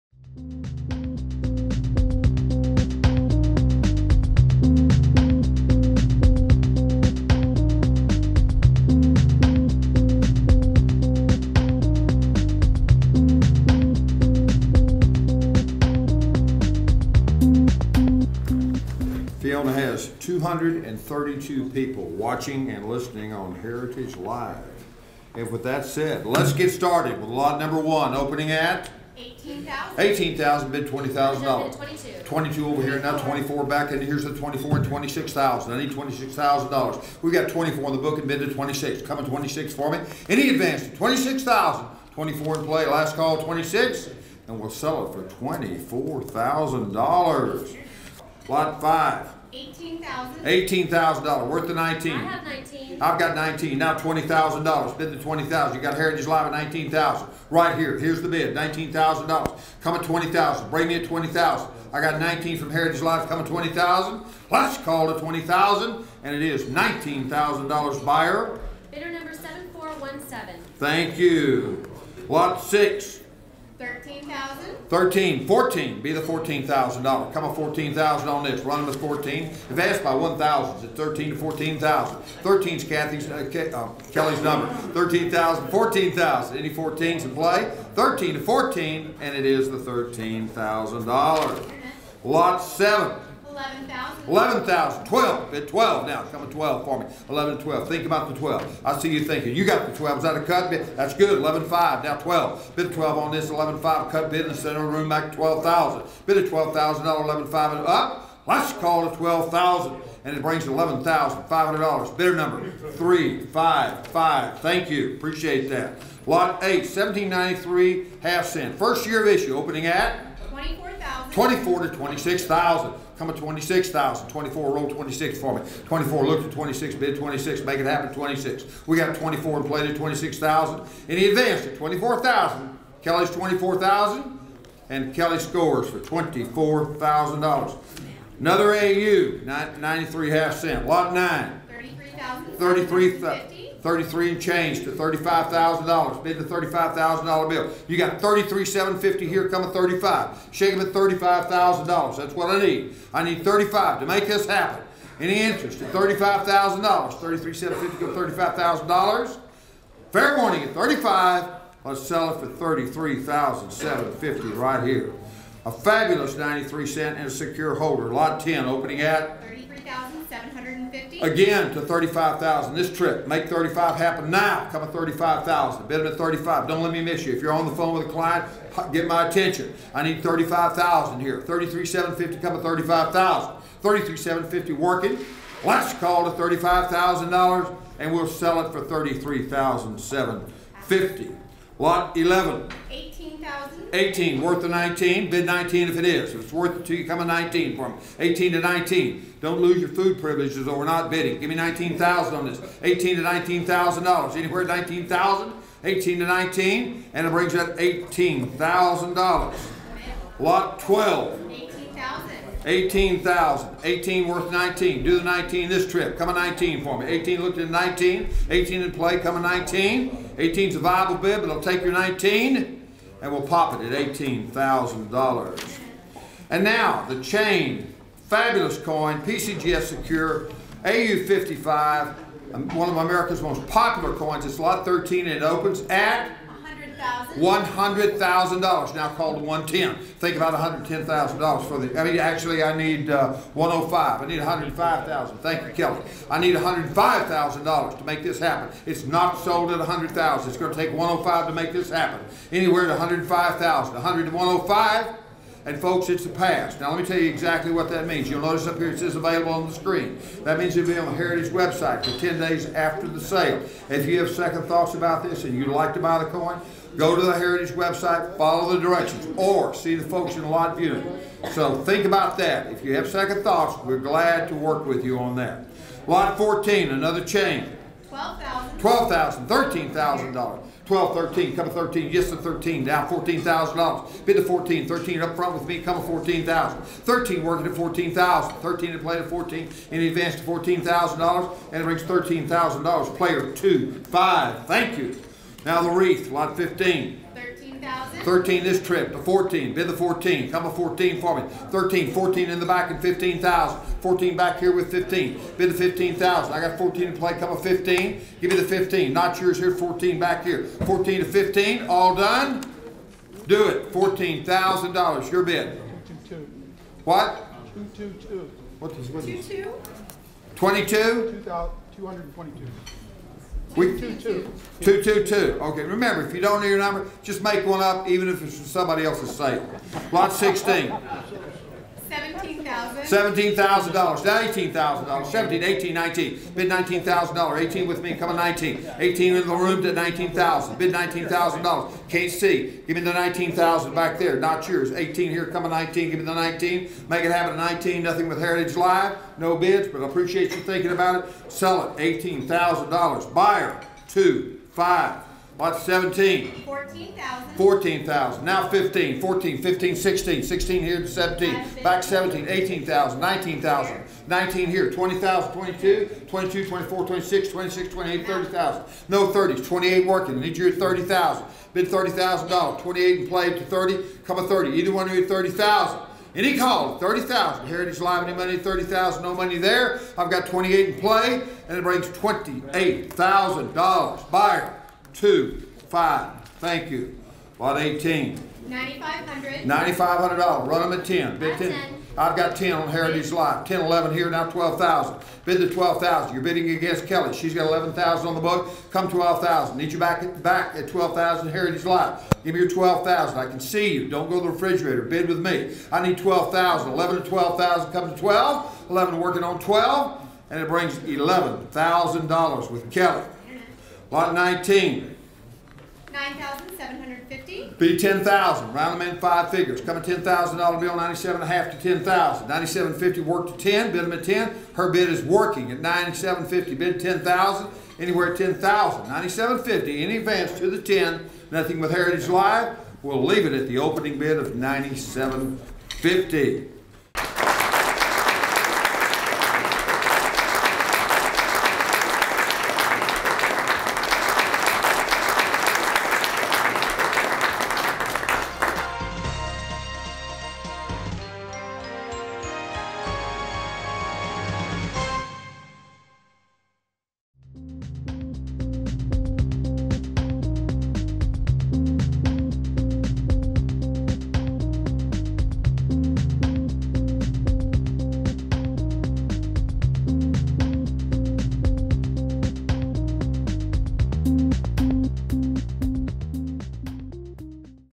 Early US Copper Sold Coins at Heritage Auctions Platinum Night, CSNS Convention
Auctioneer: Bob Merrill
Platinum Night Sales by Heritage Auctions feature some of the most rare and desirable United States coins. At this year's Platinum Night Sale held at the Central States Numismatic Society Convention in Schaumburg, IL some classic early copper coins were sold. 7:58
More news and video about coin and paper money collecting